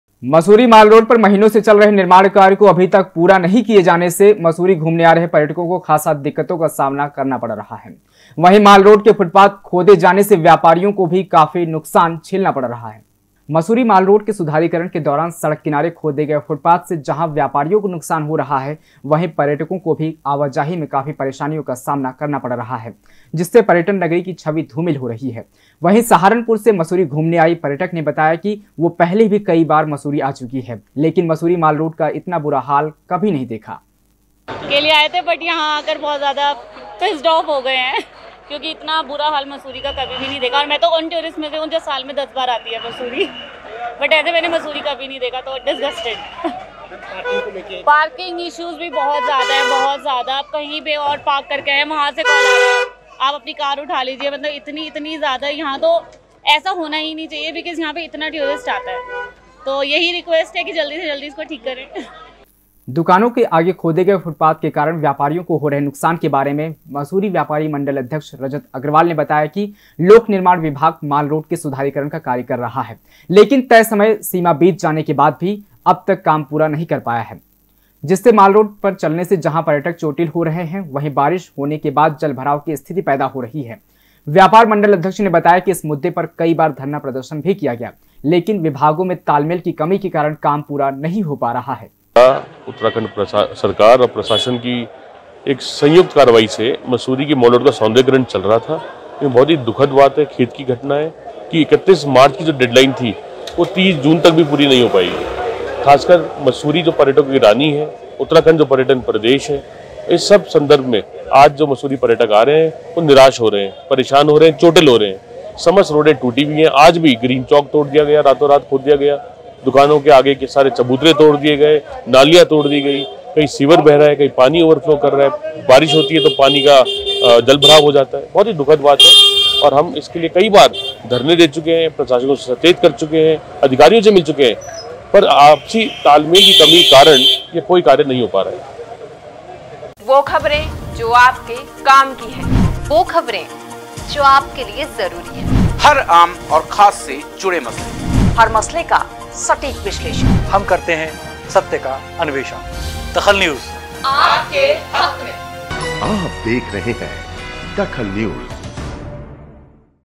DAKHAL NEWS मसूरी माल रोड के सुधारीकरण से हो रही परेशानी
मसूरी माल रोड के सुधारीकरण से हो रही परेशानी
पर्यटकों को खुदी हुई सड़कों से हो रही परेशानी